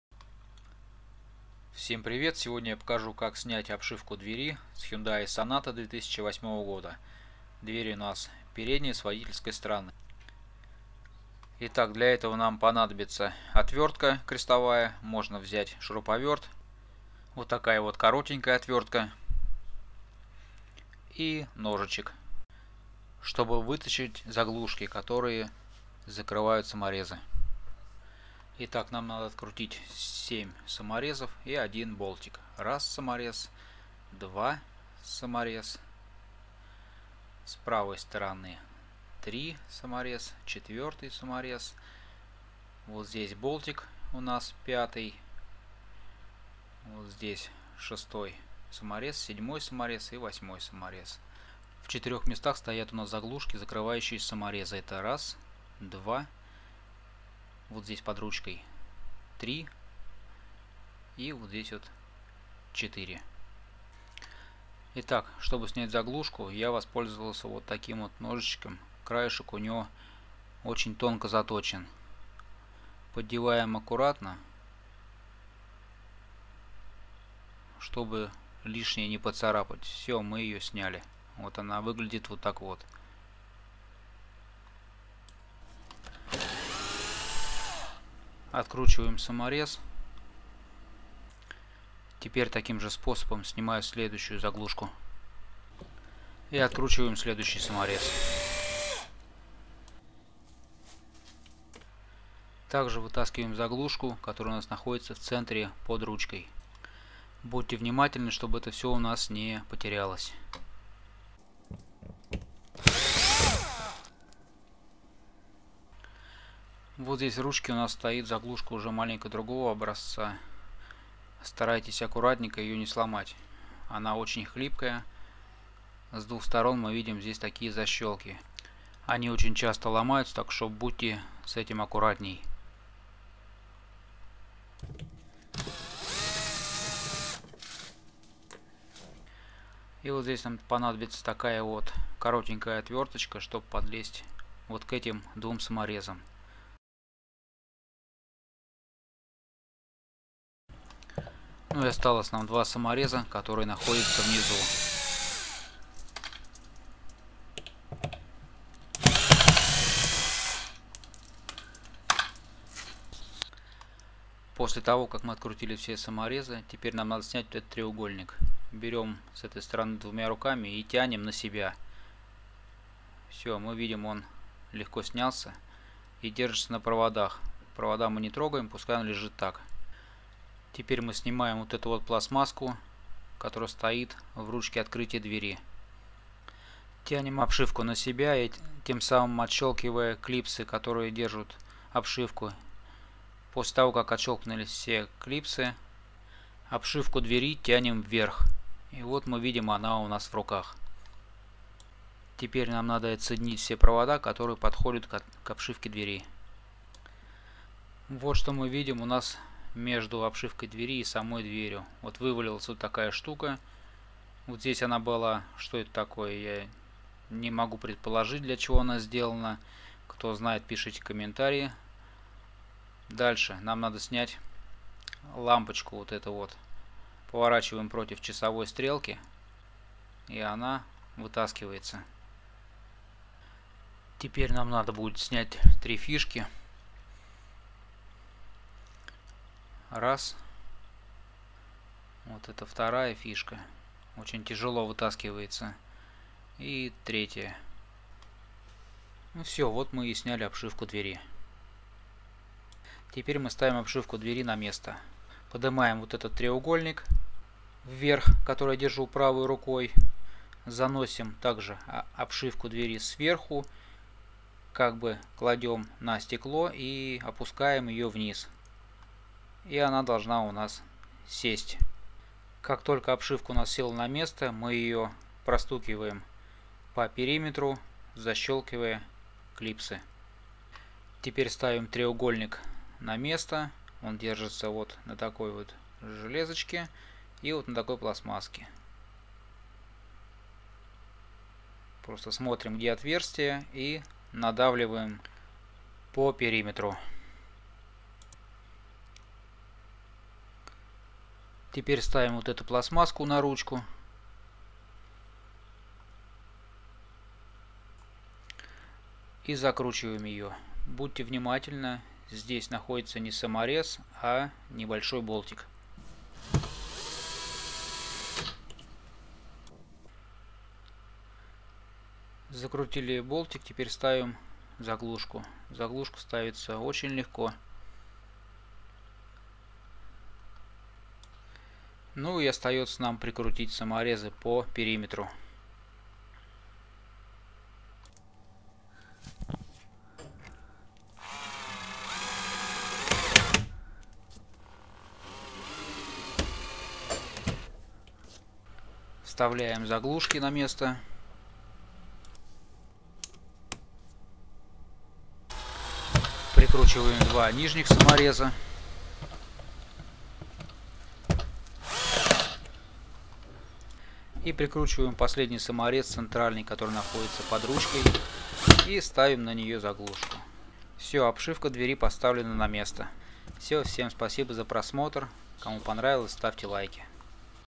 Как снять обшивку двери Хендай соната
Как снять обшивку двери  Хендай соната? Это очень, посмотрим в этом видео.